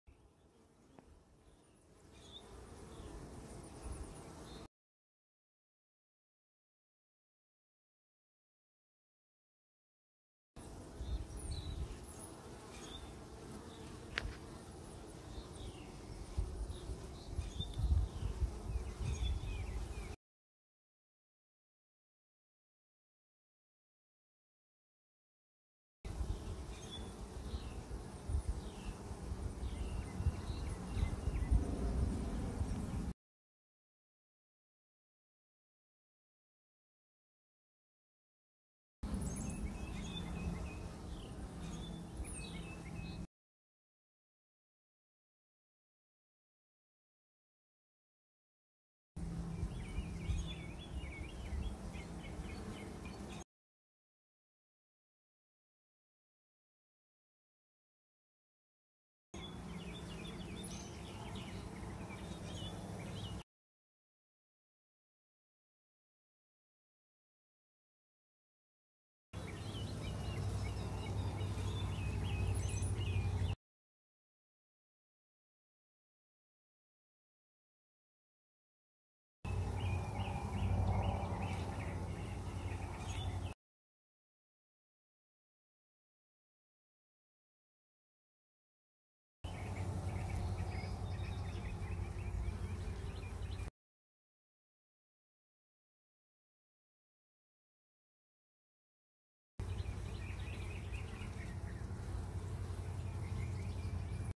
super slow motion
Triyng my new phone with slow motion and super slow motion